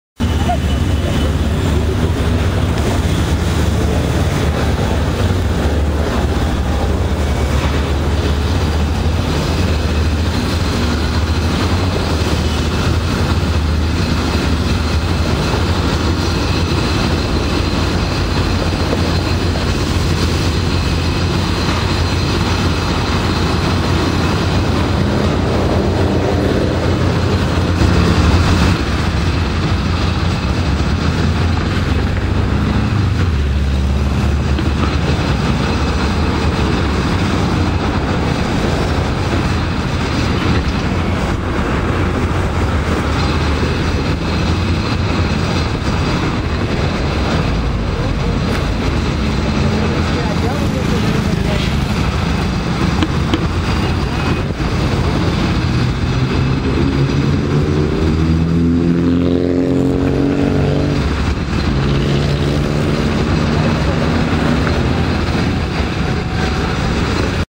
Torre Punta Reforma - Construcción - 15-05-2013
Aquí la mas reciente actualización de la torre, que va muy rápida. :D